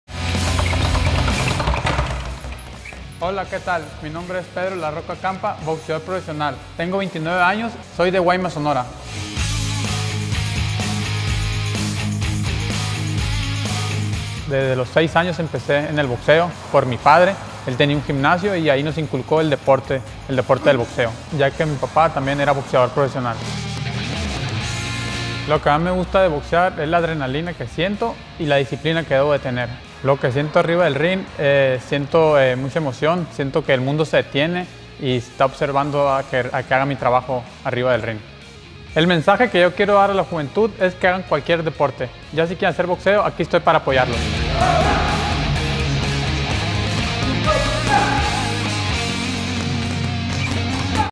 Día Internacional del Deporte
En el DíaInternacionalDelDeporte les comparto un poco de la historia del boxeador profesional Pedro "La Roca" Campa 👏🏻. ¡Orgullo sonorense!